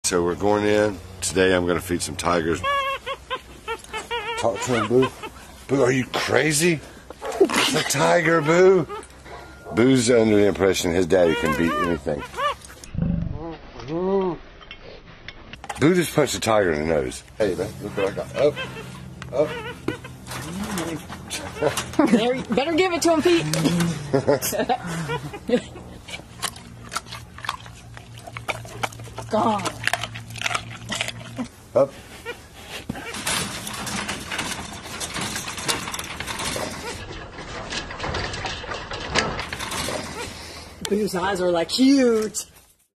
Monkey vs. Tiger!
MonkeyBoo slaps a tiger in the face!